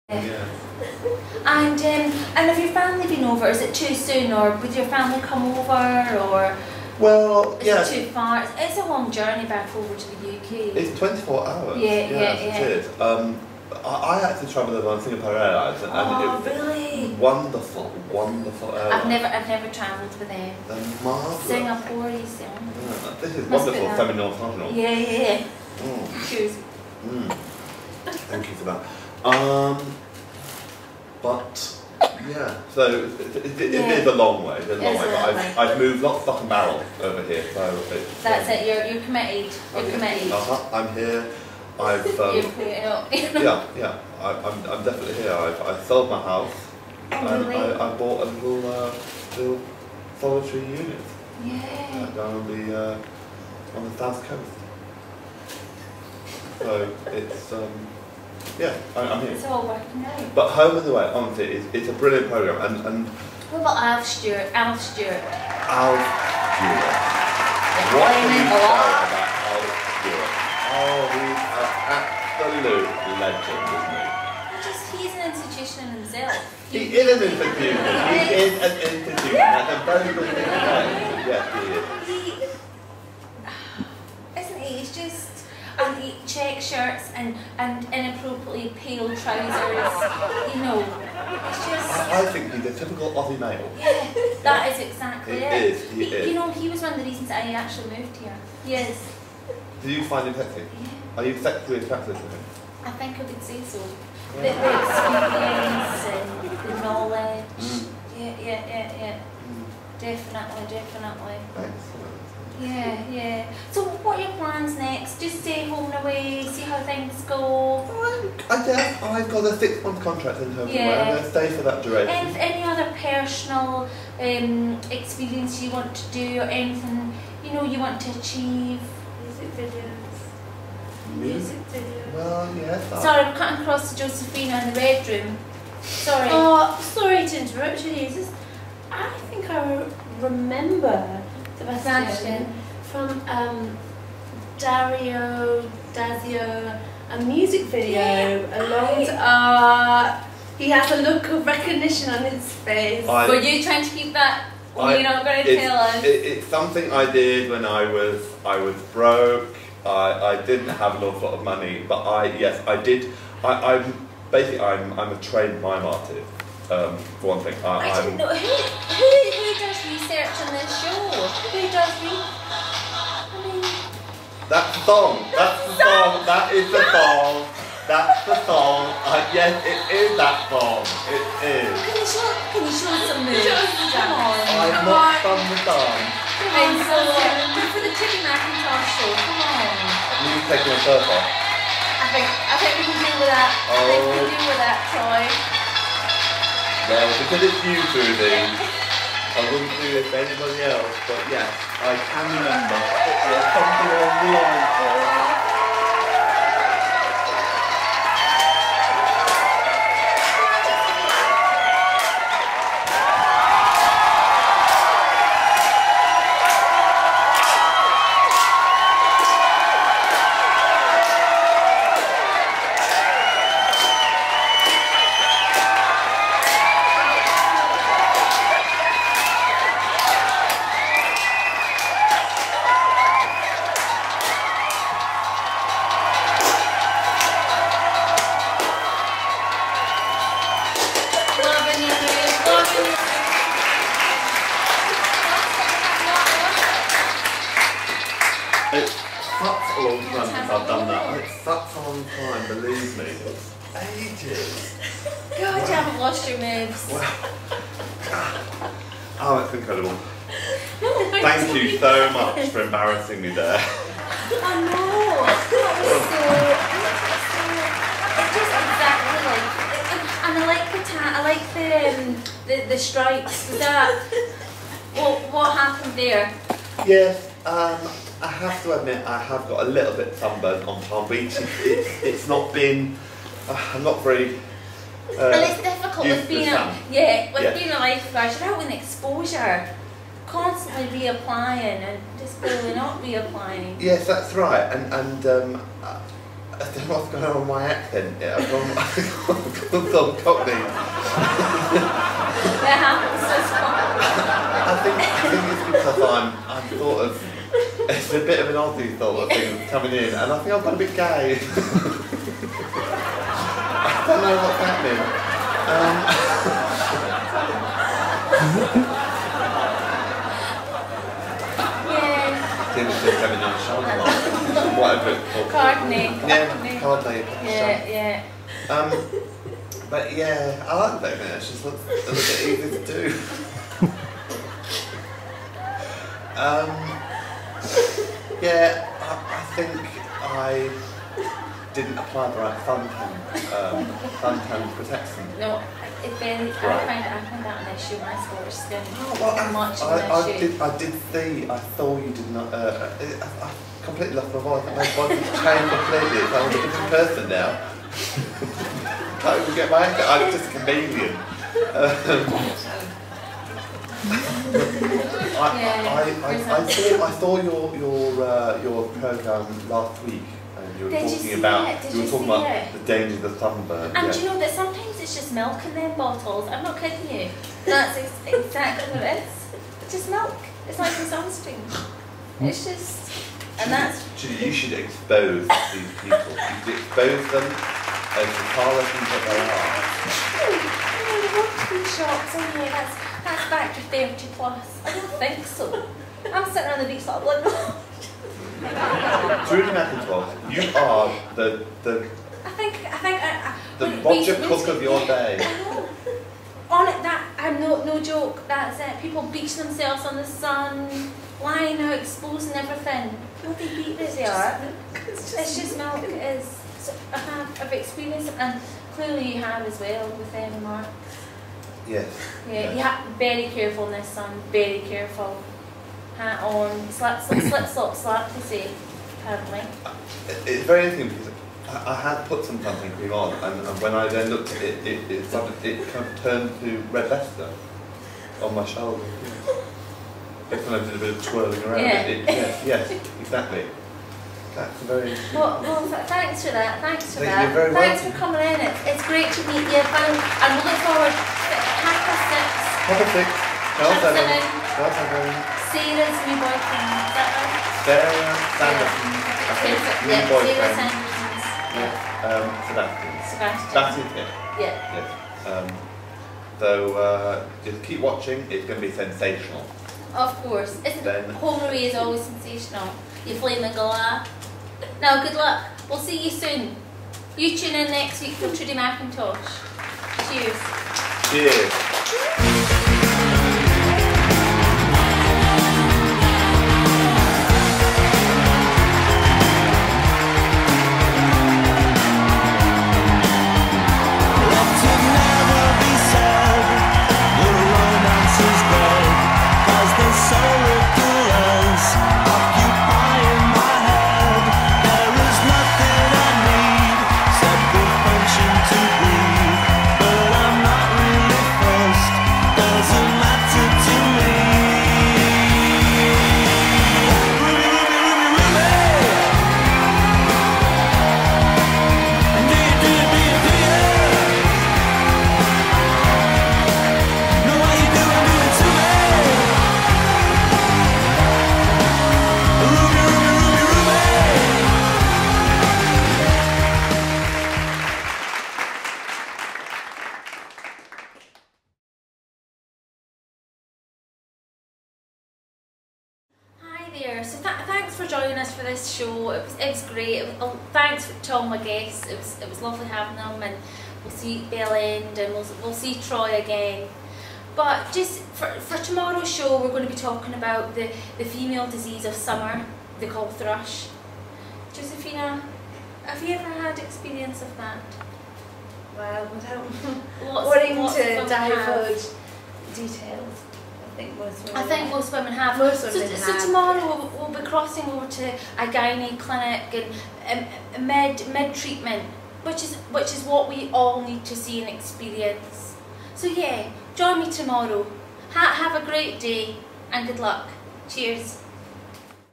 The Trudi Show 4
Scotland's own Trudi McIntosh first came to public attention 5 years ago when she publicly announced she was having an affair with Alf Stewart from Home and Away. She insisted she had moved to Australia to be near him so she could stroke his comb-over!

Following her release from the mental health institution in which she'd been incarcerated for making such bizarre claims, she applied for a job as a typist at the Channel 7 studios but went through the wrong door and soon shot to fame with a six month stint as the weather girl with the most on Channel 7's gossip and chat magazine show, This Afternoon.

Due to a phenomenal public response to her frank manner, her calm but commanding way of questioning and her ability to stay absolutely professional whilst being completely wasted, she was immediately promoted to co-host the show with Neil Taylor. But after a succession of off-air (and occasional on-air) spats, Trudi managed to frame Neil for a crime he didn't commit so that she could be the sole presenter.

Now fronting her own show on Channel 10 from her holiday home in Cairns, Trudi brings her Scottish charm to the airwaves once again. Sold all over the world, the Trudi show is set to make Ms McIntosh into a star. With the creme de la creme of star guests queueing up to be interviewed by her, she looks set to be on our screens for some time to come.

In this episode, she interviews Geordie star of stage and screen Josephina Dreamcoat, exercises her eyebrows, ears and pelvic floor with alleged lesbian exercise freak, Bel End, gets all the latest Hollywood gossip from celebrity whore, Richard Gaylord and interviews new Home and Away hunk Troy Jockstrap about his role as Sebastian Smith. Oh and if you're a fan of cameraman Steve - this episode celebrates his birthday.